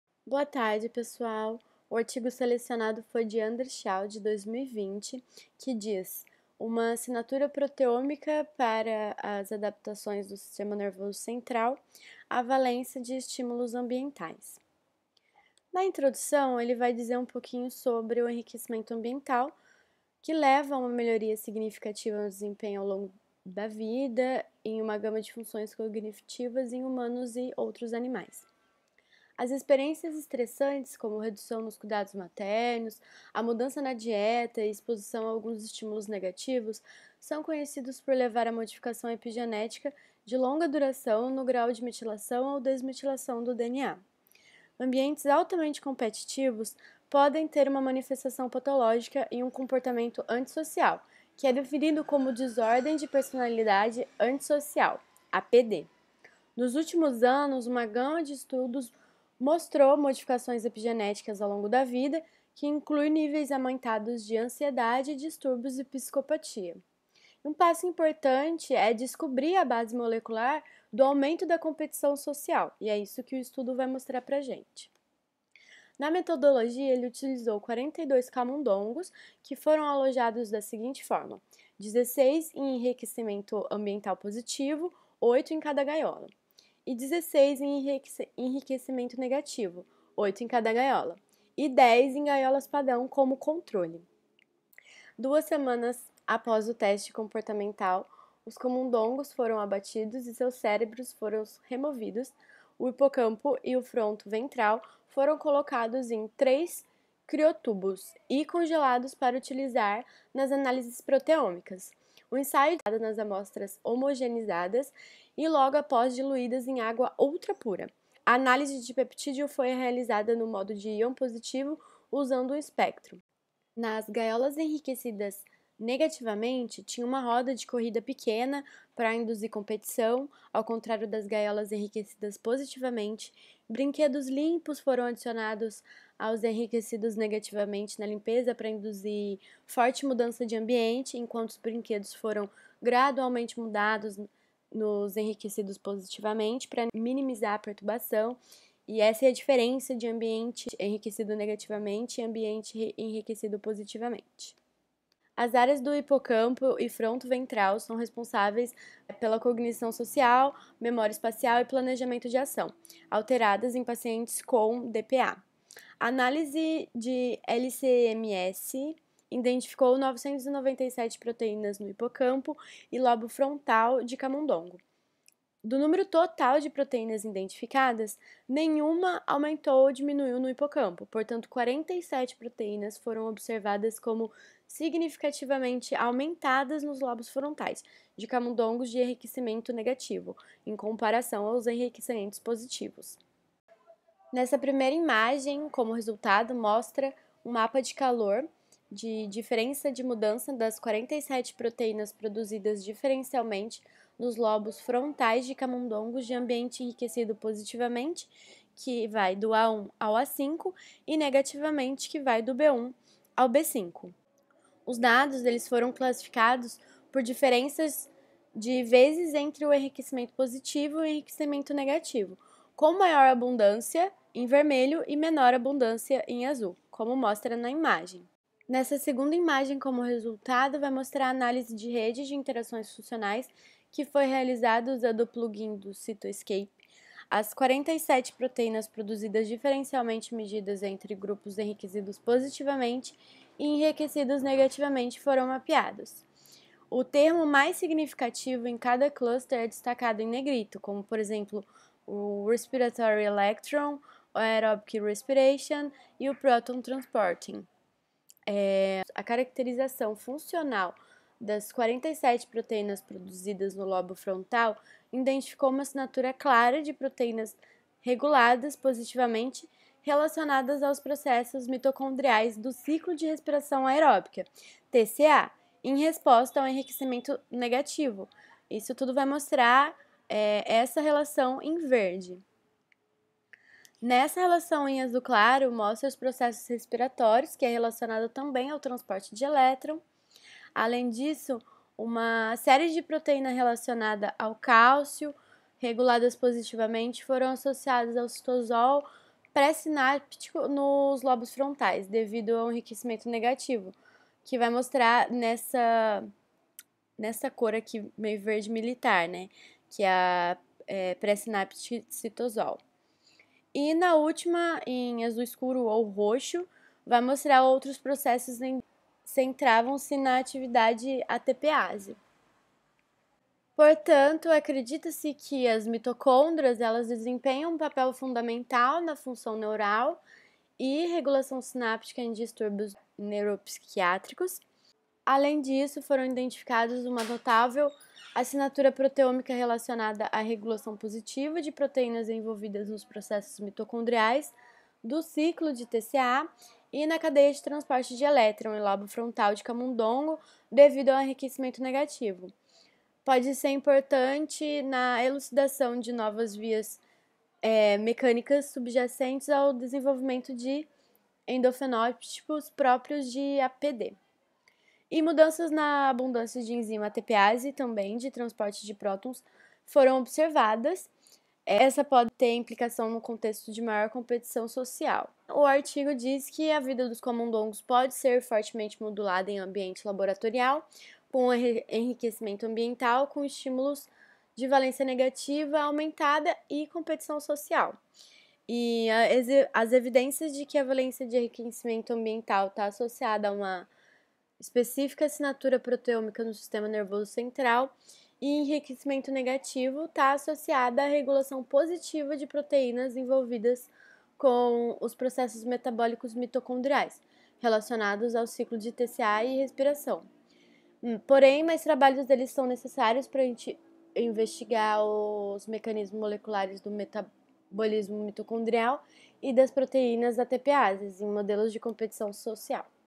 ARTIGO 3 - Isabelle Giacobini
Produzido por Isabelle Balsalobre Giacobini, graduando em Ciências Biológicas da UFRJ

Artigo: SHAW, Andrew et al. A proteomic signature for CNS adaptations to the valence of environmental stimulation. Behavioural Brain Research, v. 383, p. 112515, 2020.

Esse vídeo foi planejado como parte da avaliação da disciplina MCB488 - Proteômica e transcriptoma do curso de Bacharelado em Ciências Biológicas da UFRJ. Professora responsável Manuela Leal da Silva